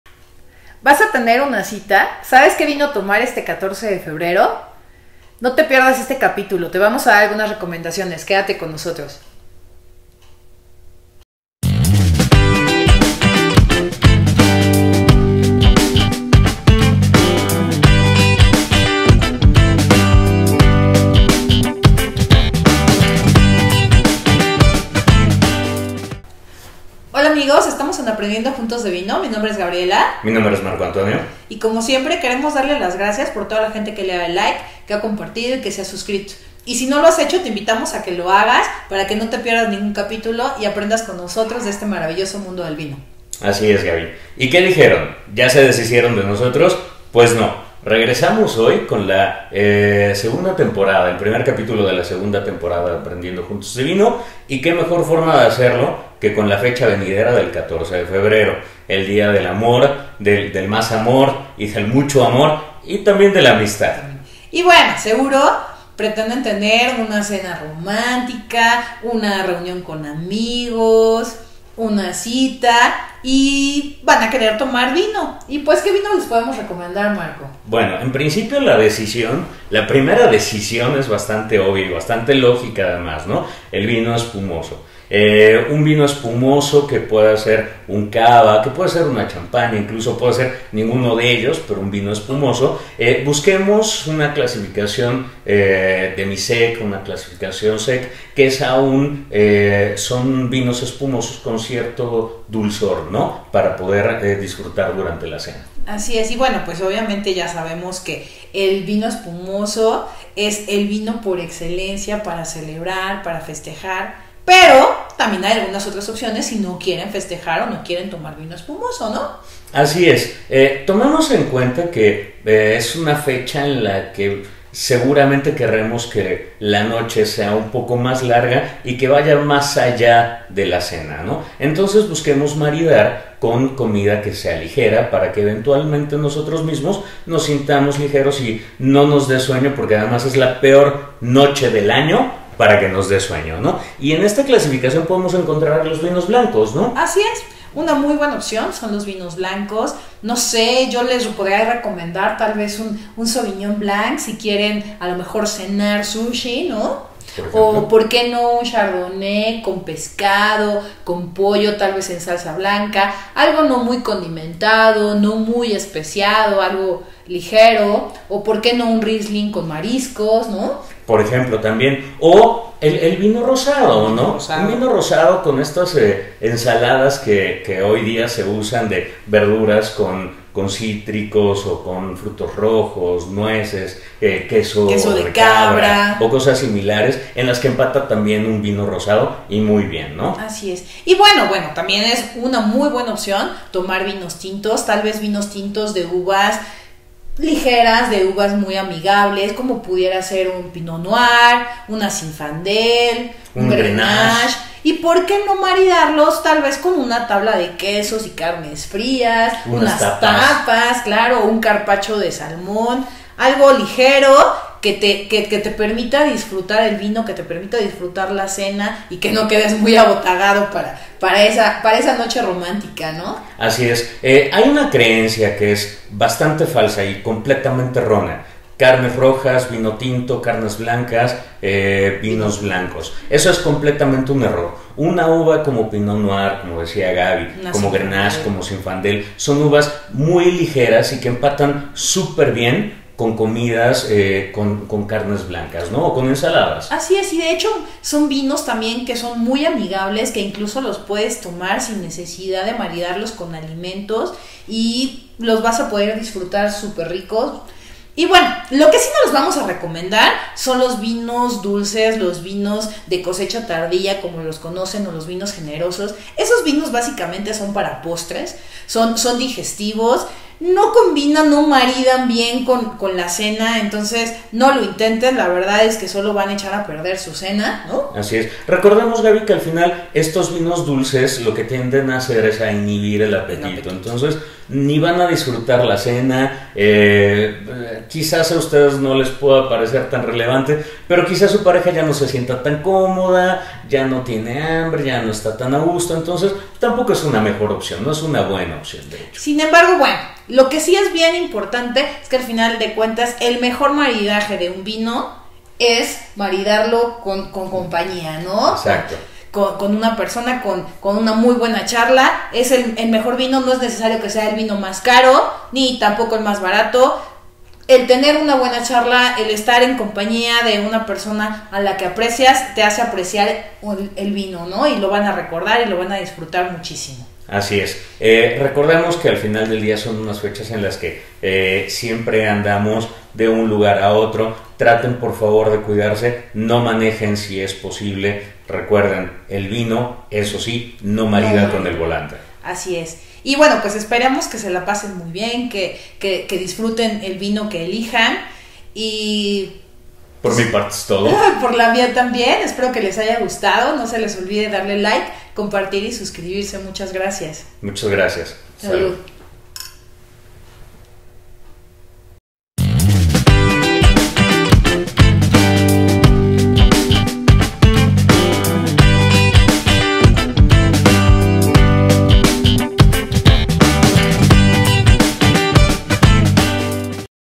Capítulo 19 - Vinos para una cena Romántica
¿Tendrás una cena romántica?, ¿Te gustaría saber como combinarlo con la comida?, en el primer capitulo de nuestra segunda temporada te damos algunos consejos para esa cena especial.

Gracias.